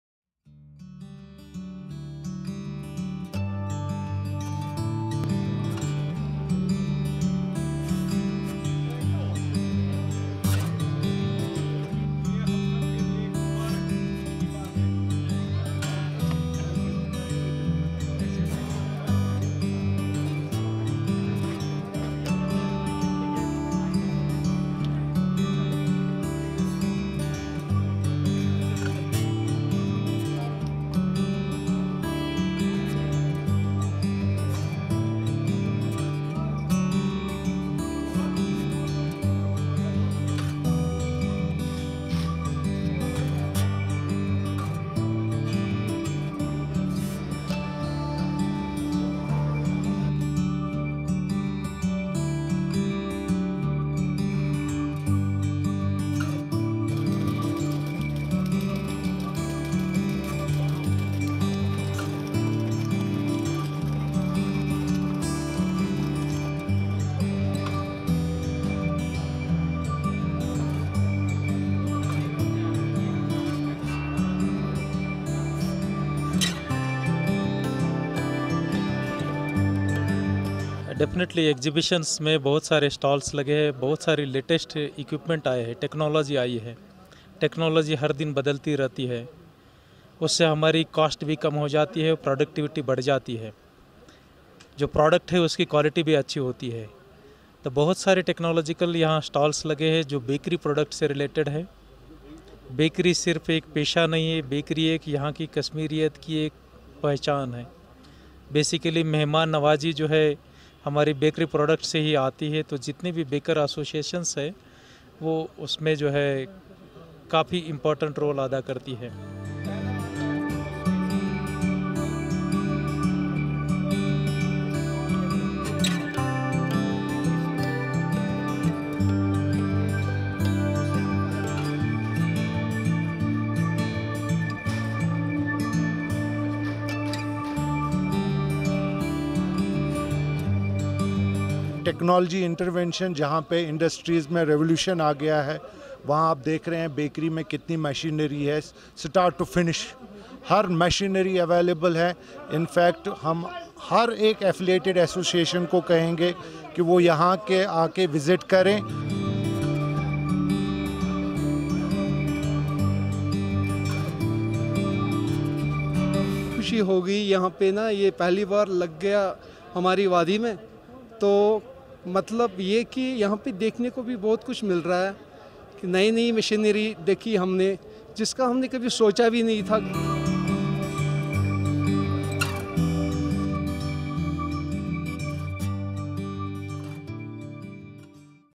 Kashmir: Exhibition on bakery and food processing machinery held in Srinagar
Srinagar, June 10 (ANI): A three-day exhibition was held at Srinagar by Kashmir Olampia company in association with the UT government of J&K. It includes industrialists, manufacturers of bakery, cold chain and food processing machinery from different parts of the country. This exhibition was an opportunity for the stakeholders to directly interact with the machine manufacturers who come to exhibit their machinery in this exhibition. The purpose of the exhibition is to create awareness and create an opportunity for the stakeholders to see the required machines physically in front of them. Around 50 stalls have been installed in which, industrialists manufacturing various machines for bakery, confectionery and cold chains have displayed their machines directly with the buyers. It created more industrial avenues in Kashmir and generated more employment in the valley. The exhibition got a good response from all over Kashmir and opened more industrial employment in Kashmir. (TS)